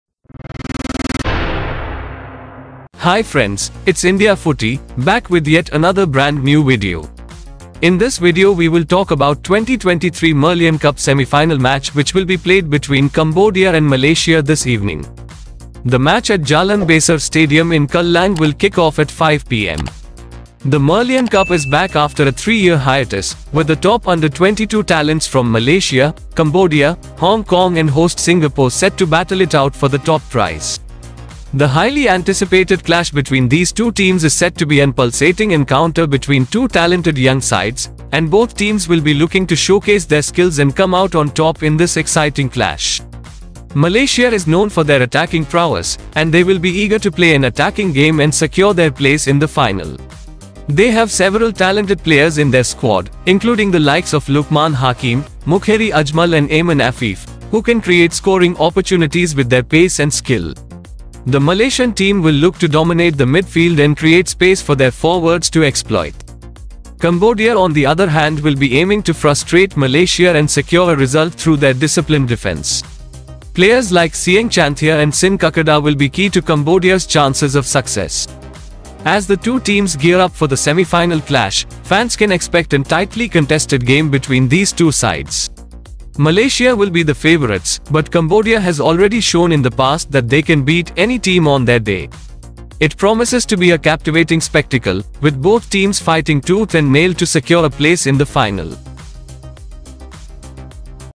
Malaysia vs Cambodia | Merlion Cup 2023
In this video we talk about Merlion Cup 2023 semifinal between Malaysia vs Cambodia.

Malaysia vs Cambodia live will be played at Jalan Besar Stadium.

Malaysia vs Cambodia u22 will kick off at 5 PM.